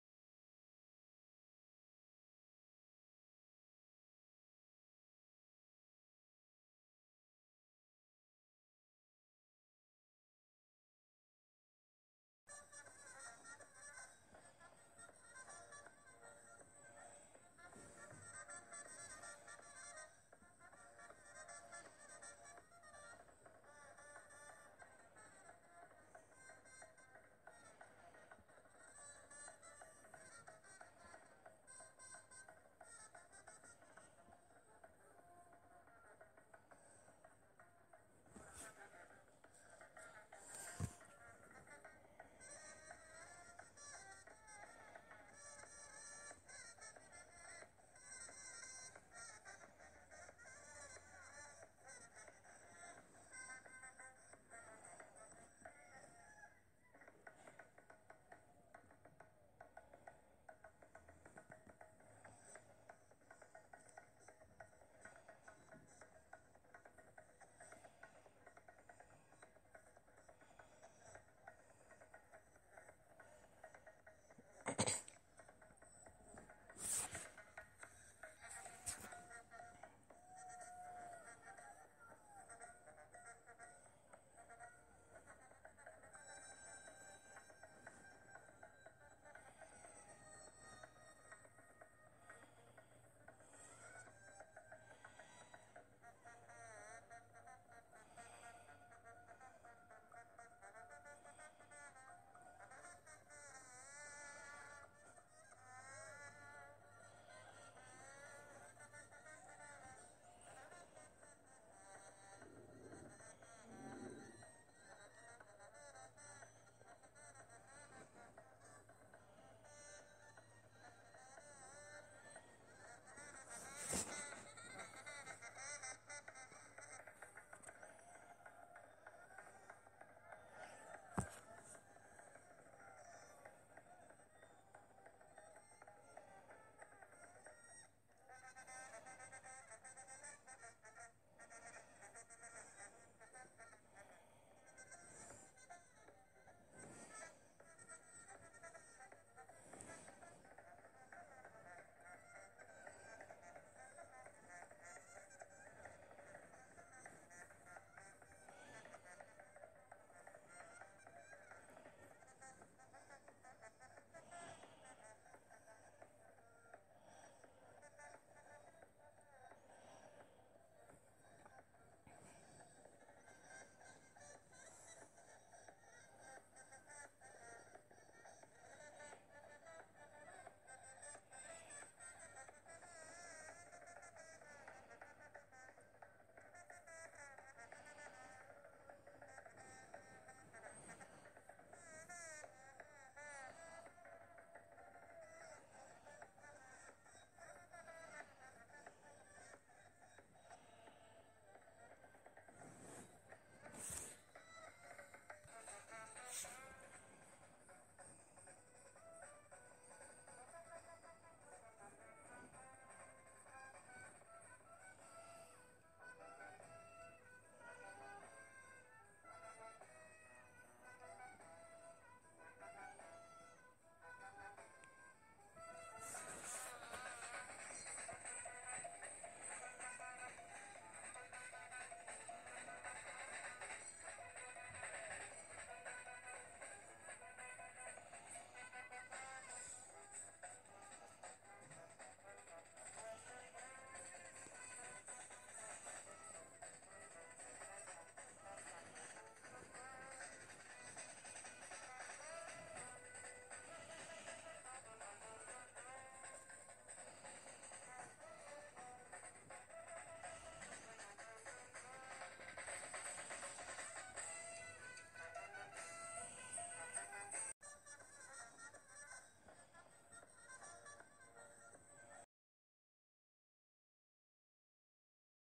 Especial carnaval
Subo esta recopilación de vídeos míos de los gigantes como el especial carnaval porque fui a las 18:59 a Torrejón porque allí deberían estar los gigantes y aparecieron cuando me fui a mi casa y no pide ir a grabar los de alcala